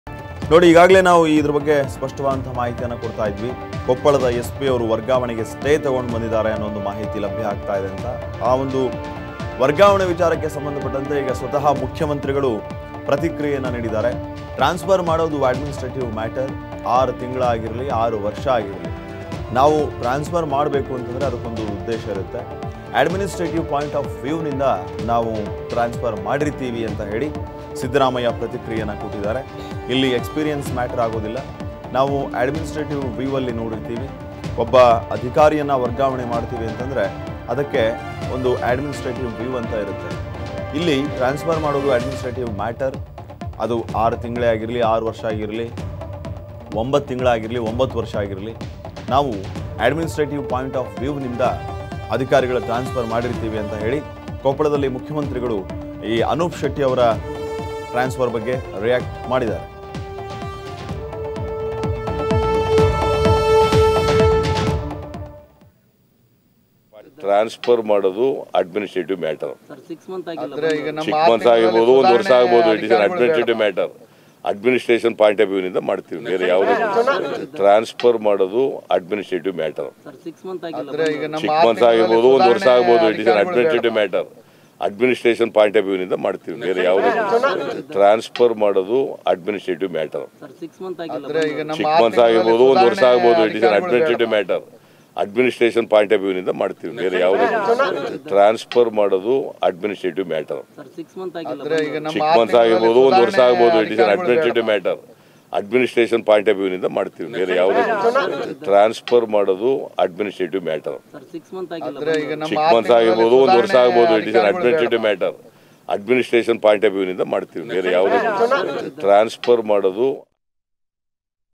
S P Anoop Shetty ವರ್ಗಾವಣೆ ವಿಚಾರಕ್ಕೆ ಸಿ.ಎಂ.ಸಿದ್ದರಾಮಯ್ಯ ಹೇಳಿದ್ದೇನು ಗೊತ್ತಾ..!?
S P Anoop Shetty ವರ್ಗಾವಣೆ ವಿಚಾರಕ್ಕೆ ಸಿ.ಎಂ.ಸಿದ್ದರಾಮಯ್ಯ ಹೇಳಿದ್ದೇನು ಗೊತ್ತಾ..!?"ಟ್ರಾನ್ಸಫರ್ ಮಾಡೋದು ಅಡ್ಮಿನಿಸ್ಟ್ರೇಟಿವ್ ಮ್ಯಾಟರ್"...